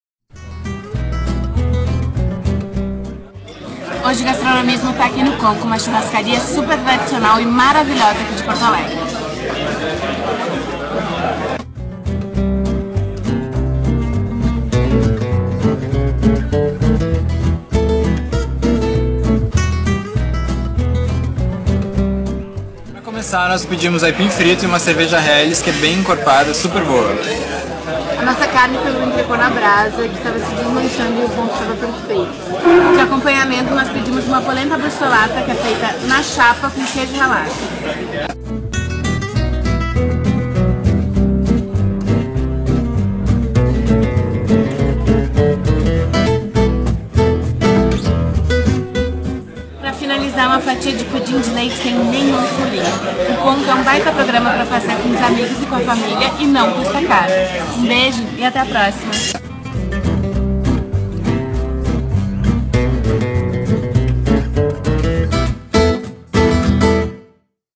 Tastemade EATS: Komka
Este vídeo, faz parte de uma série de episódios com dicas de lugares legais para comer em Porto Alegre, feitos com o aplicativo para iPhone da Tastemade. Faça também os seus vídeos. Compartilhe e descubra comidas imperdíveis